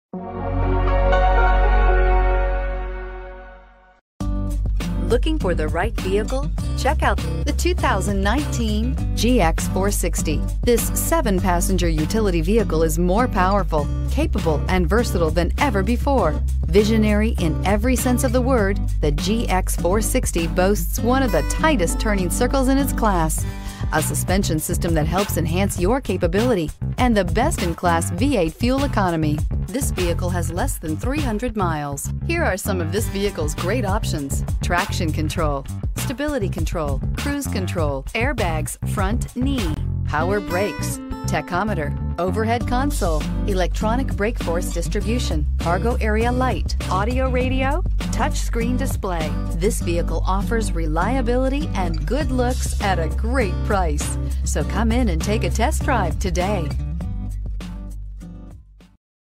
2019 Lexus GX 460 for sale in Raleigh NC
2019 Lexus GX 460 for sale in Raleigh NC - Stock # RGT282
Year: 2019
Engine: 4.6L V-8 cyl
Transmission: 6-Speed Automatic with Sequential Shift ECT
MPG Range: 15/18
Exterior Color: Nebula Gray Pearl
Interior Color: Sepia
Address:
VIN: JTJBM7FX2K5229292